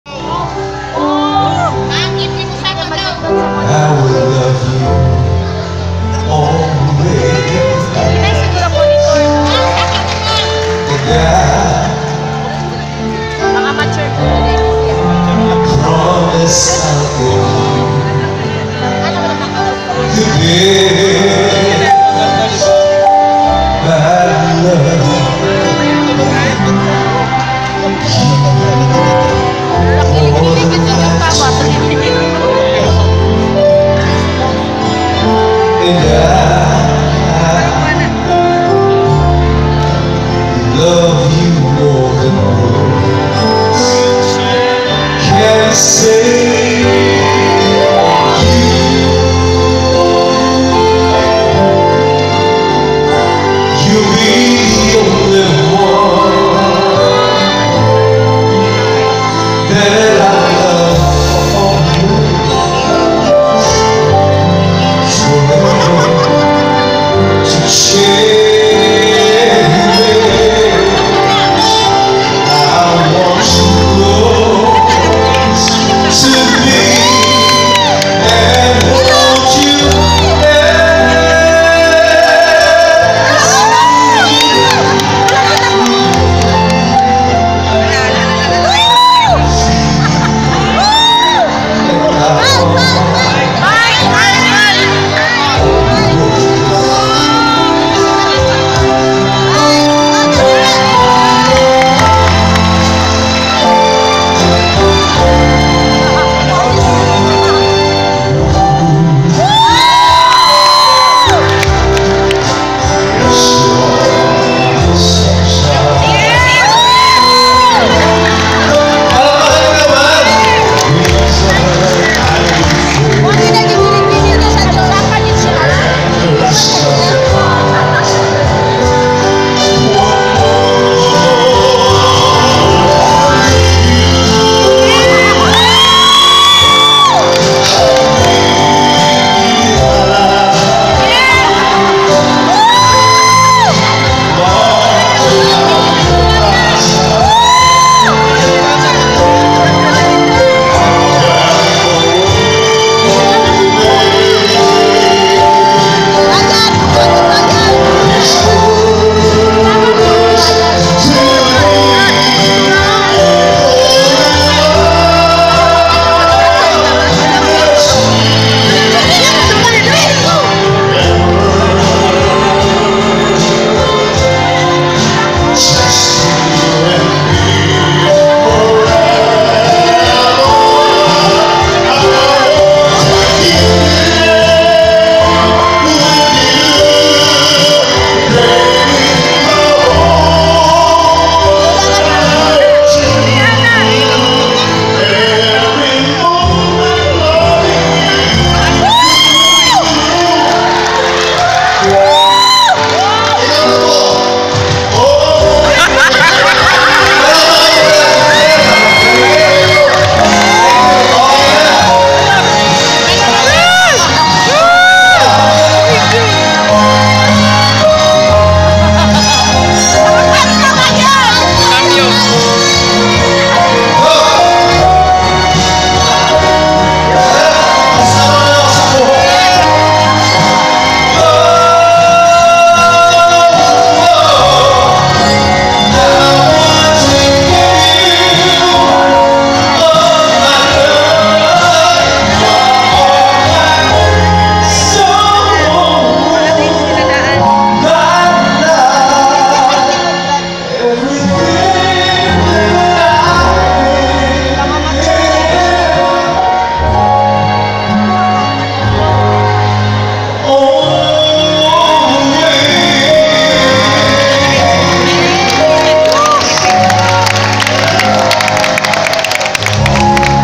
Marco Sison at 10th Bulawan Festival/19th Founding Anniversary in Compostela Valley Province
During Bulawanong Pasundayag, Culmination of 19th Founding Anniversary and 10th Bulawan Festival, March 8, 2017